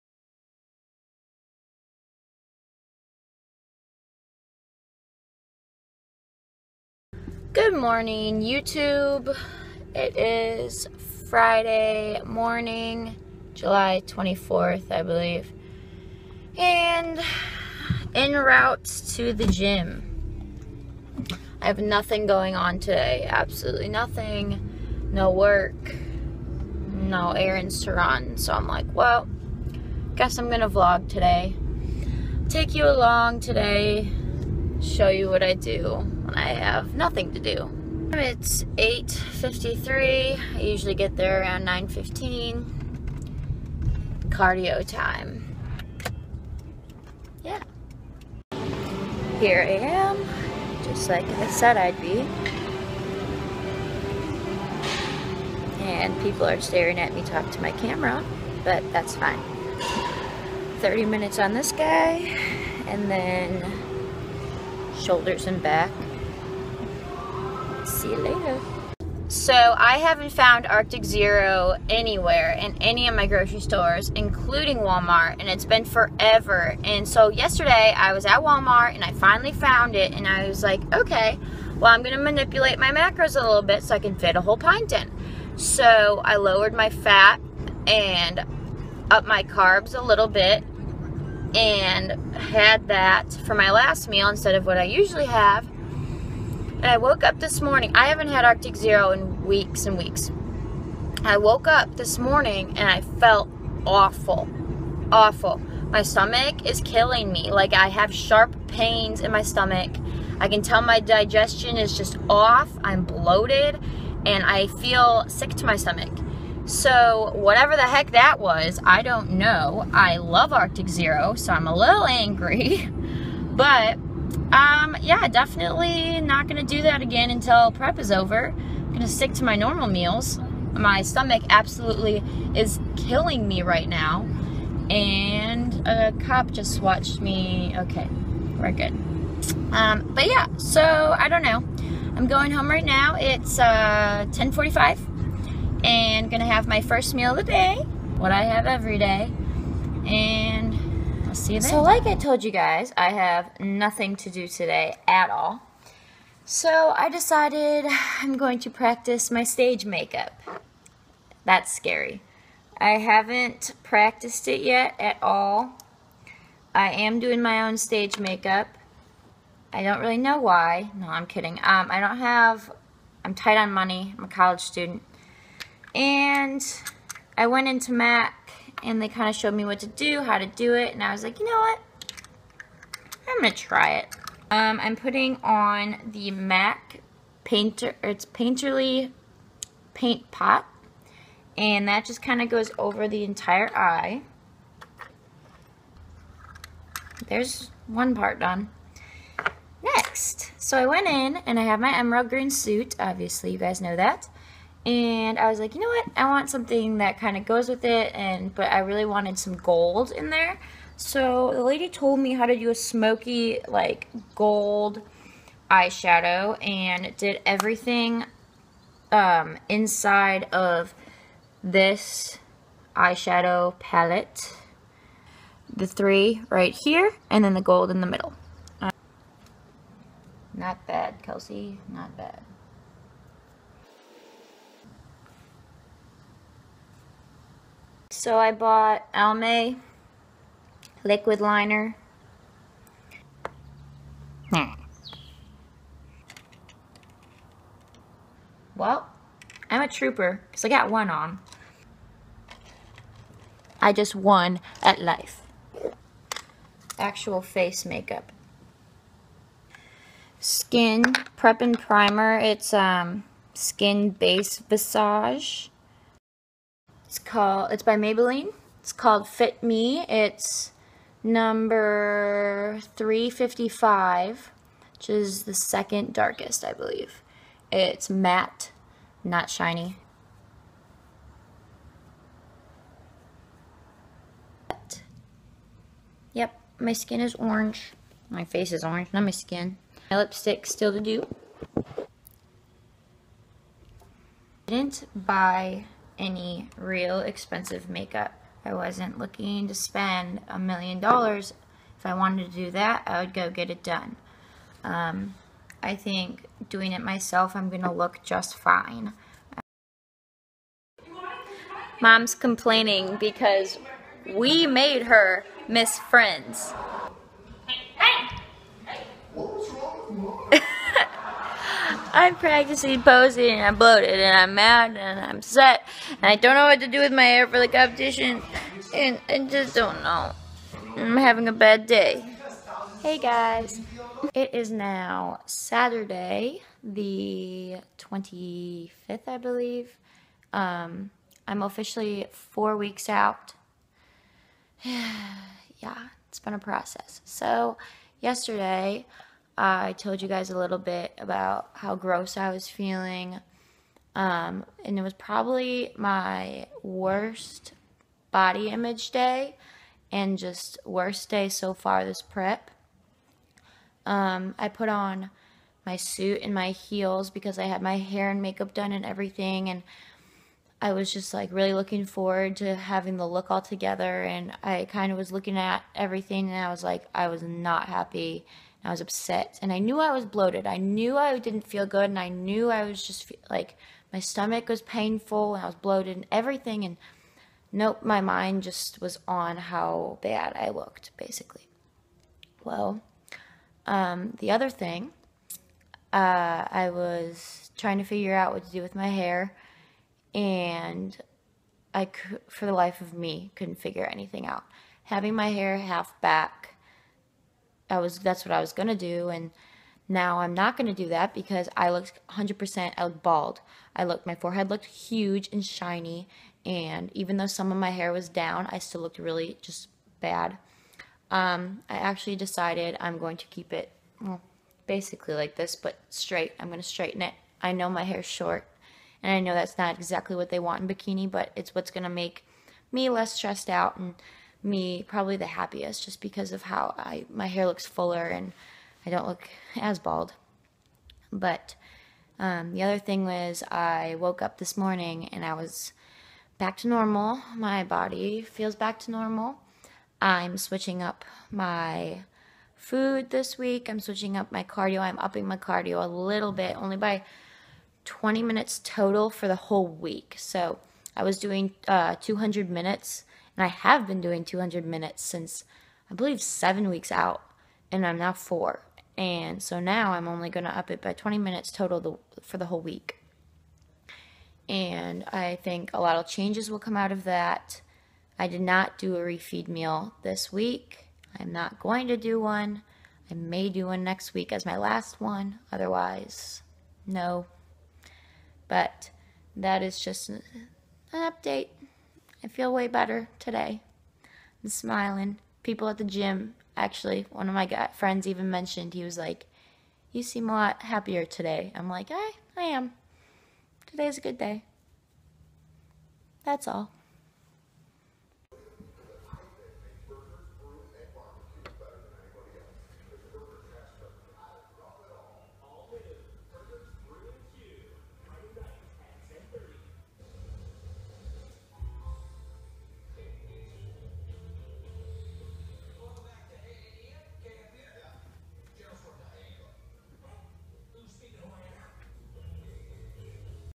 4 Weeks Out - Stage Makeup - Prep Feels
129.4lbs and 4 weeks away from bikini competition